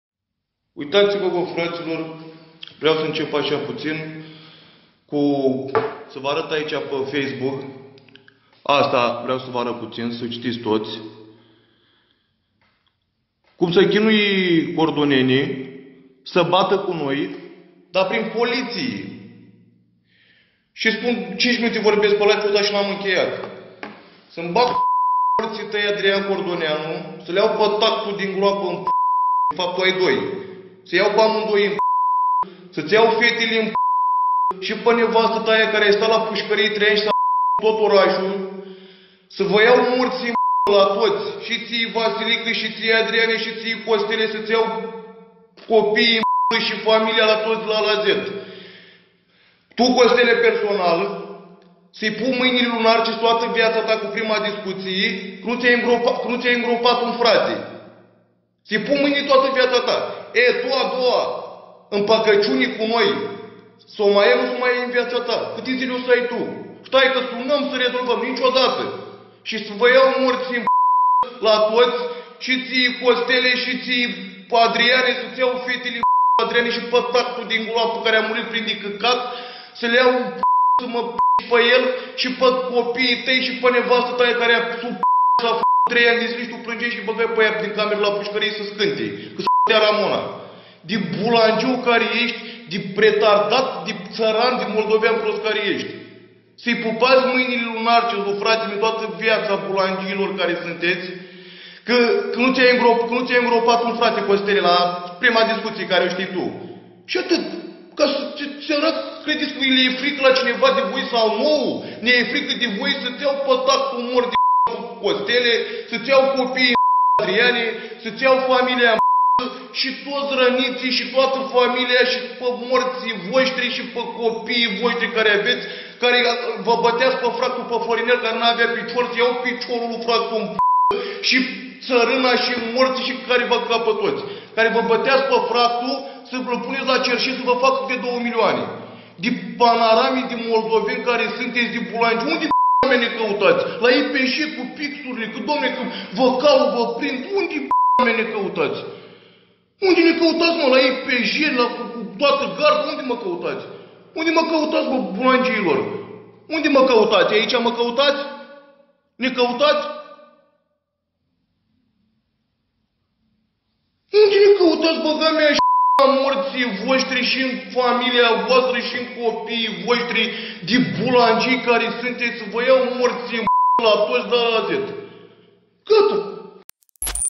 Interlopul Vandam il injura pe Adrian beleaua
adrian corduneanu,vandame,iasi